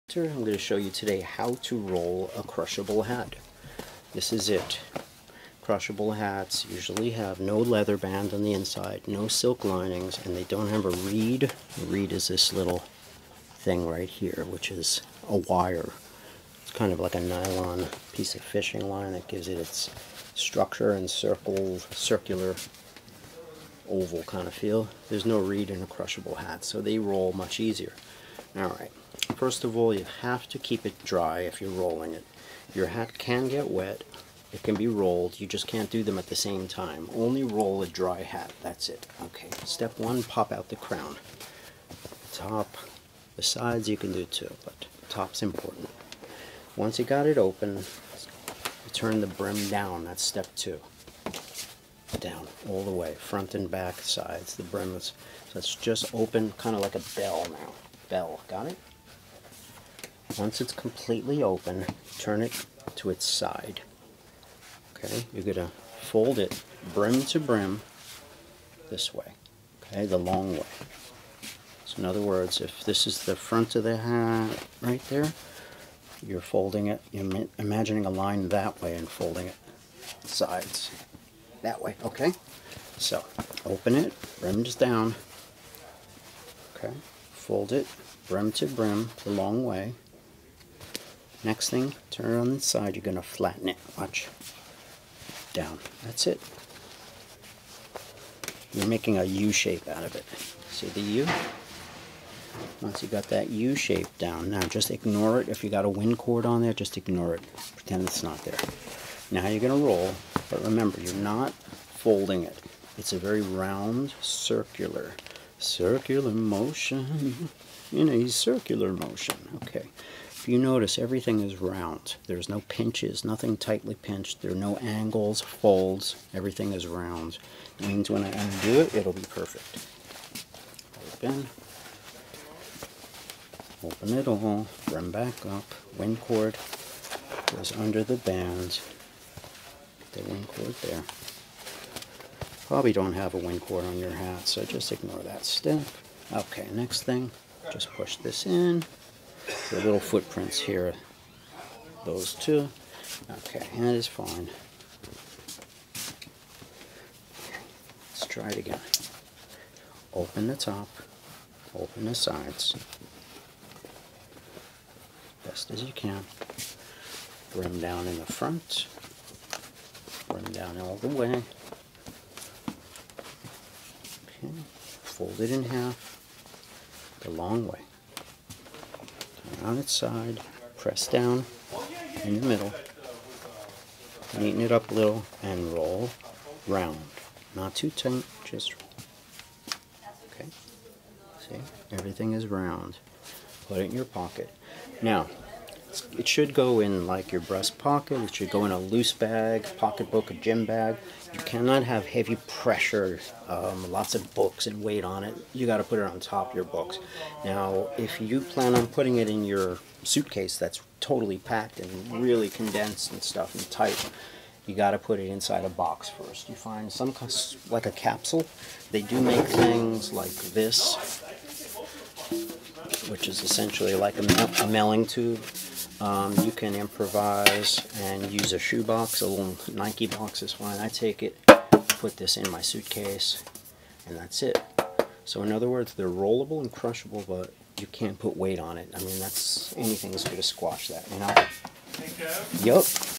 How To Roll A Crushable Hat, ( Felt Or Straw )
Sincerely,
Rolling a hat is easy, it must be dry,
don’t roll it too tightly, and don’t FOLD it, ROLL it...
Once u get this down,after a few times, u will be able to do this while running for the bus,it gets easier.🤠
I had to end this one abruptly, sorry about that..
one of my colleagues was asking me for assistance